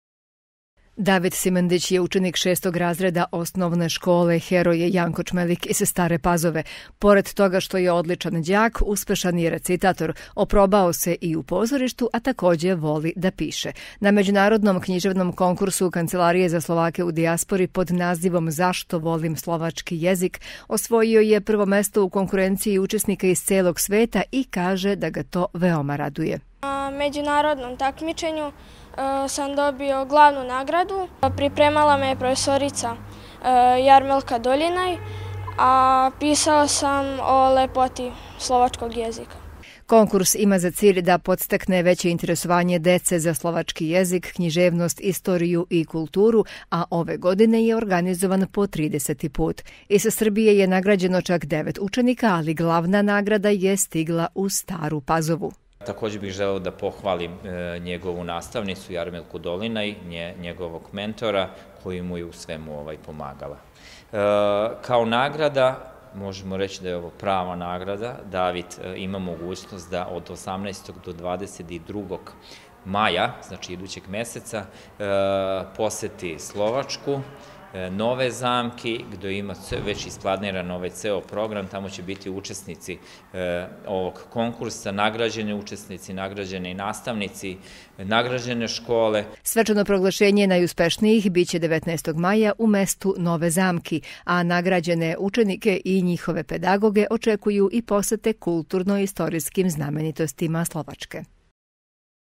Davidu Simendiću iz Stare Pazove prva nagrada iz Bratislave
Na takmičenjima učenici pored osvojenih nagrada, stiču i samopouzdanje, a samo učešće na bilo kakvom takmičenju ili konkursu predstavlja veliki izazov.  Osvojena nagrada donosi radost a u Staru Pazovu u ruke Davida Simendića stigla je prva nagrada sa svetskog literarnog konkursa.